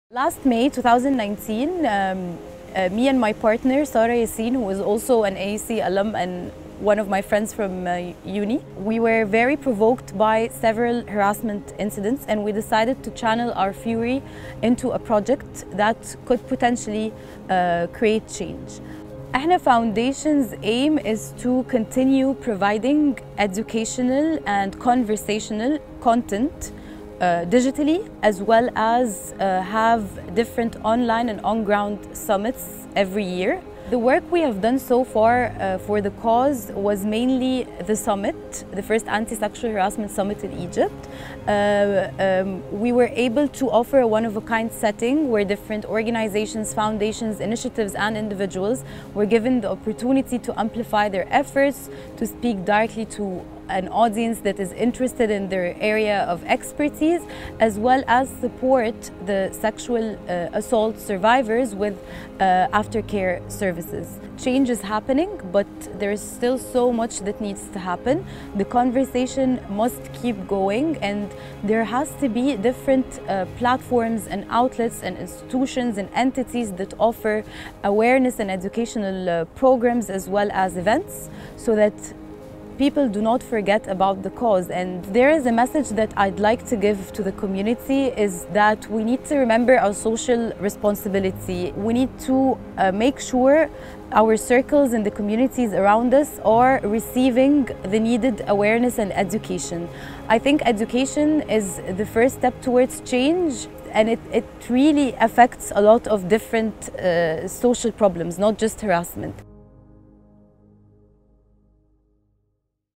Dania Younis '12, Co-founder E7na Foundation
Dania Younis '12 co-founder of E7na Foundation talks to us about her project and shares her views on the issue of sexual harassment.